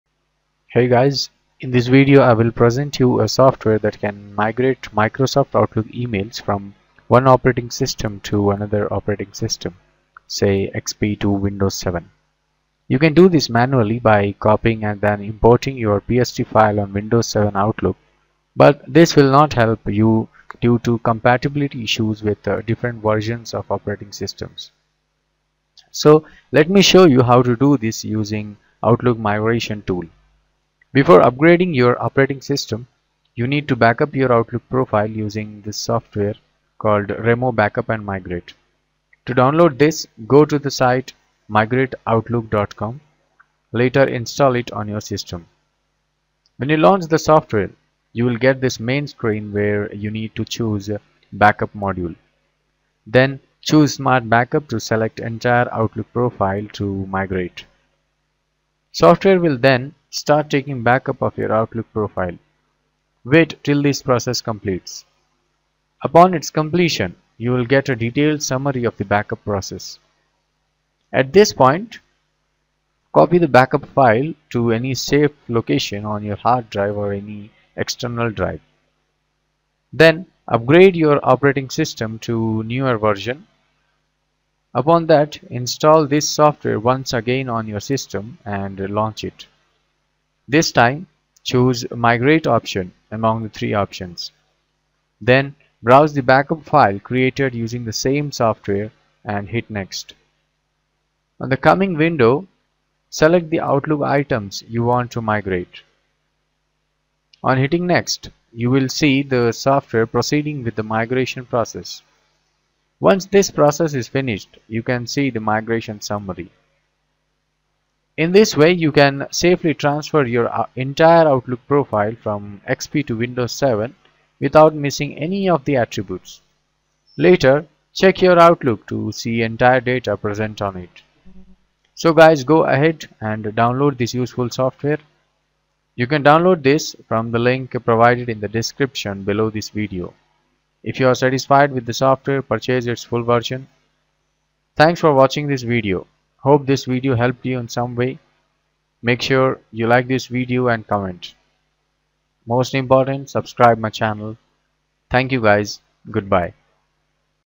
Transfer Outlook Emails from XP to Windows 7
In this video, I will present you a software that can migrate Microsoft Outlook emails from one operating system to another operating system, say XP to Windows 7.

You can do this manually by copying & then importing your PST file on Windows 7 Outlook, but this will not help you due to compatibility issues with different versions of operating systems.

So let me show you how to do this using Outlook migration tool.

Before upgrading your operating system, you need to backup your Outlook profile using this software called Remo Backup & Migrate. To download this, go to the site "migrateoutlook.com". Later, install it on your system.

When you launch the software, you will get this main screen where you need to choose Backup module.

Then, choose Smart Backup to select entire Outlook profile to migrate.

Now choose the location to save the backup file.

Here, you can use Scheduler & other options if required or else move to the next screen.

Software will then start taking backup of your Outlook profile. Wait till this process completes.

Upon its completion, you will get a detailed summary of the backup process.

At this point, copy the backup file to any safe location on your hard drive or any external drive.

Then, upgrade your operating system to newer version.

Upon that, install this software once again on your system & launch it.

This time, choose Migrate option among the three options.

Then, browse the backup file created using the same software & hit Next.

On the coming window, select the Outlook items you want to migrate and hit Next.

On hitting next, you will see the software proceeding with the migration process. Once this process is finished, you can see the migration summary.

In this way, you can safely transfer your entire Outlook profile from XP to Windows 7 without missing any of the attributes. Later, check your Outlook to see entire data present on it.

So guys go ahead and download this useful software.

You can download this software from the link provided in the beginning of this description.

If you are satisfied with the software, purchase its full version.

Hope this video helped you in some way. Make sure you like this video and comment.